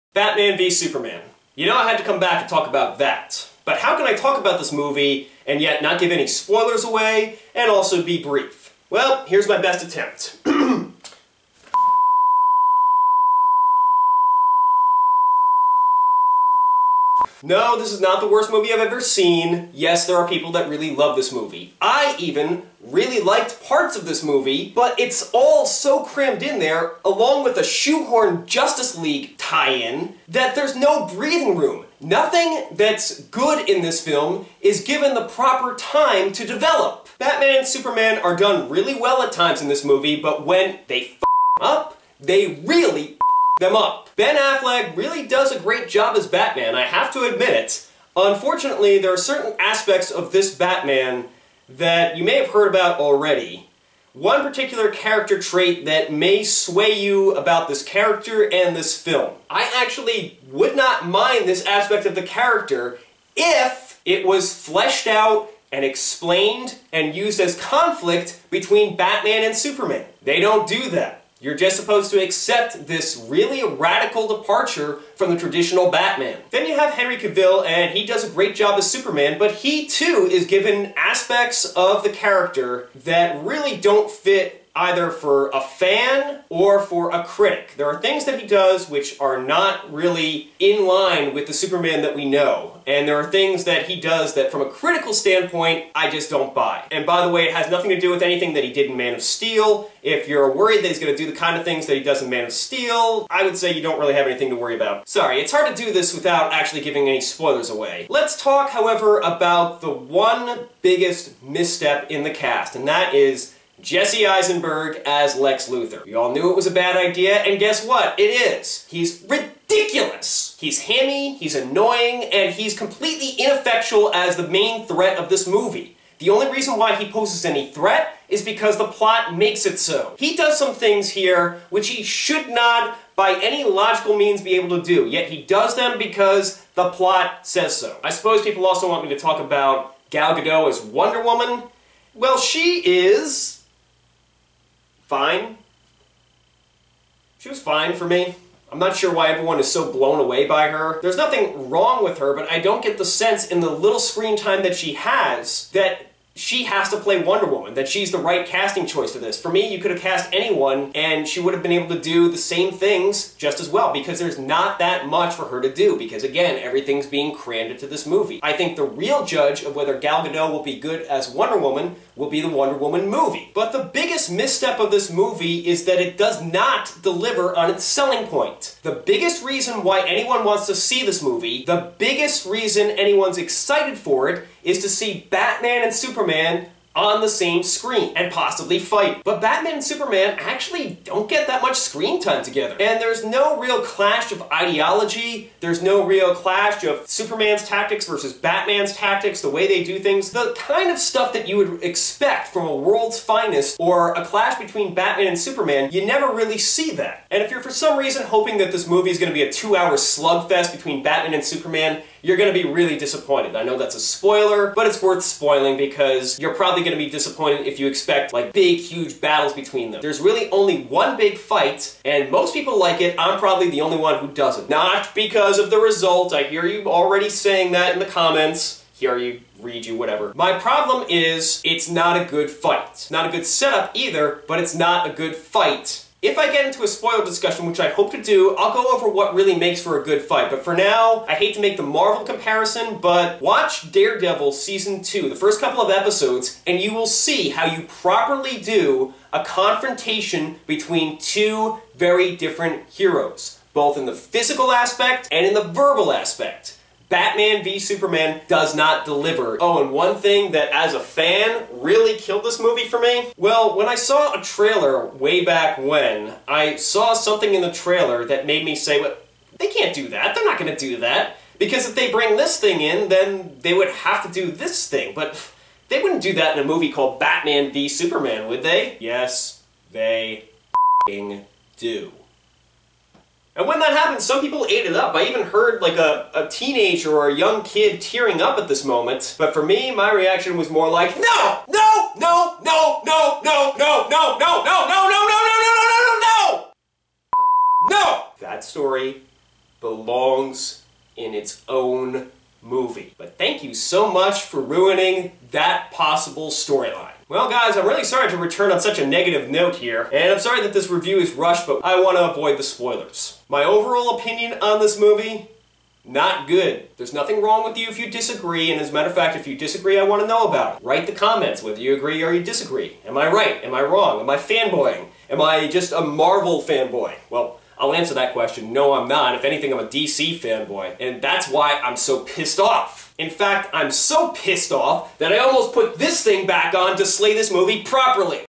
Batman V Superman Review: No Spoilers
This is a new experiment in style to see if I can make my current movie reviews a little more interesting, cause I don't have time to put the armor on. Any suggestions to make this better are welcome